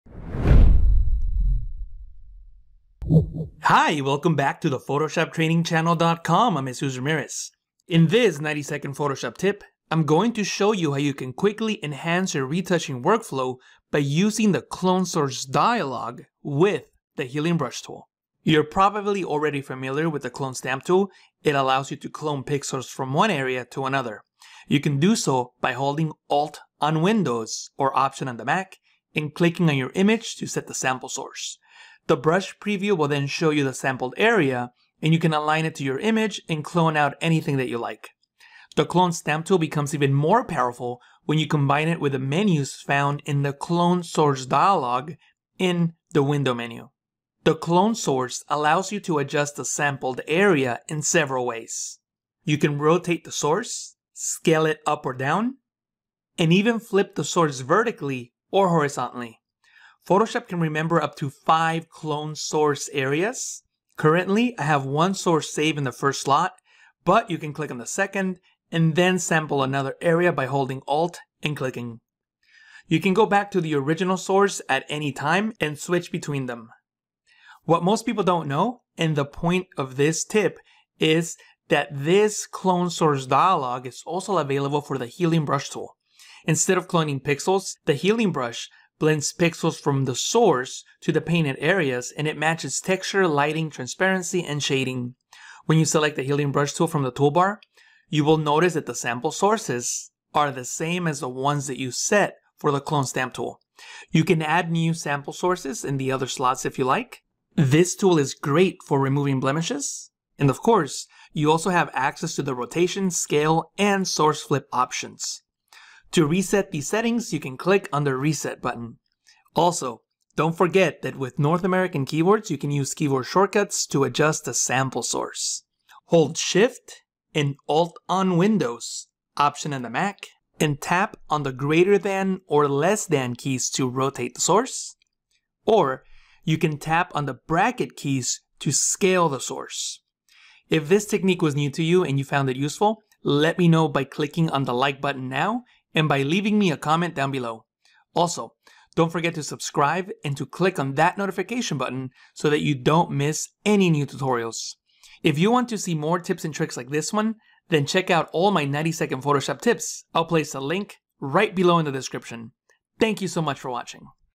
Best Healing Brush Tool Trick Ever (WORKS INSTANTLY!) 90-Second Tip #10
In this tutorial, you will learn how you can quickly enhance your retouching workflow by Using the Clone Source dialog with the Healing Brush Tool.